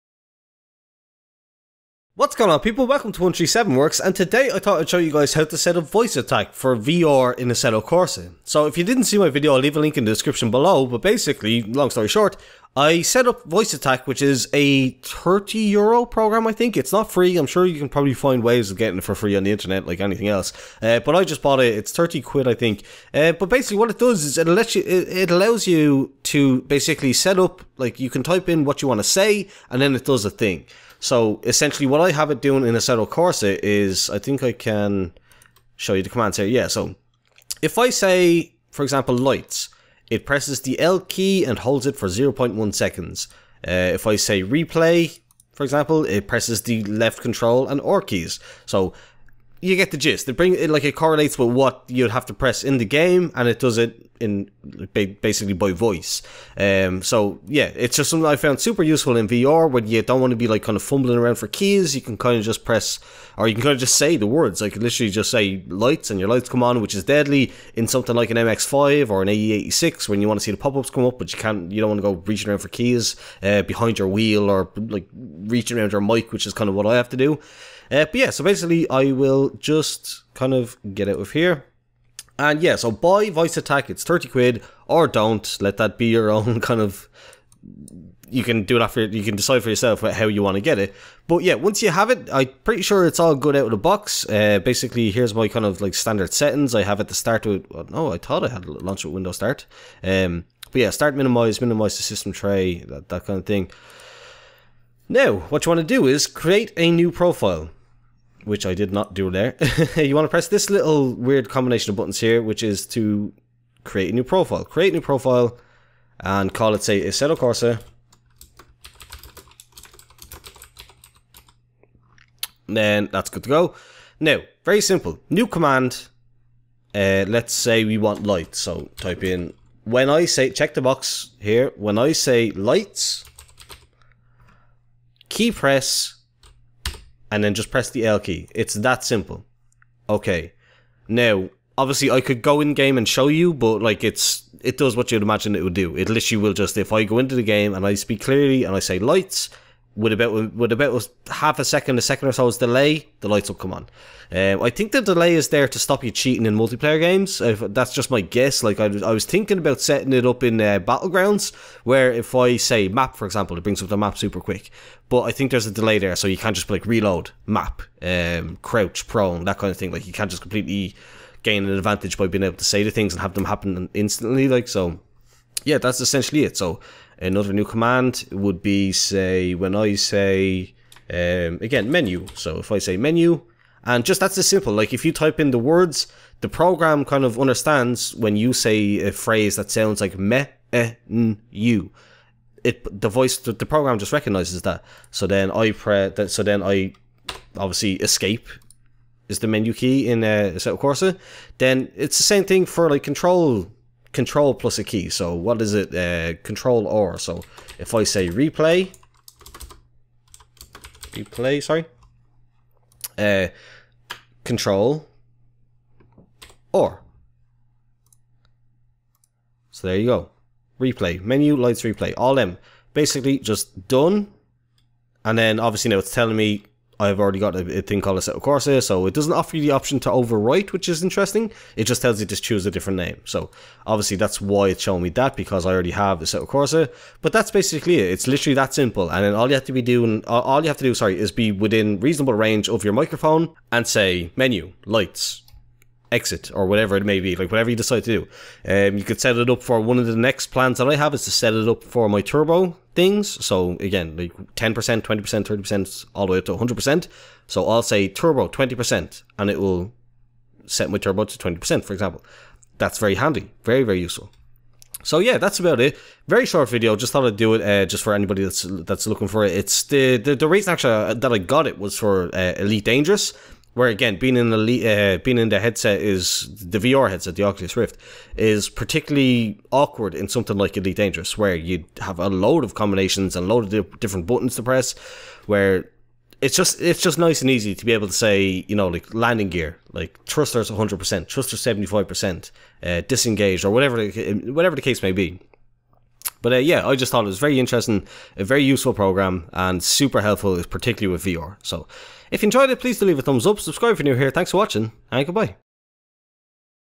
How to set up Voice Attack for Assetto Corsa in VR!
This program, while not free is super usefull when in VR, and works for any/all games and programs! It's been a godsend to me for my AC VR stuff!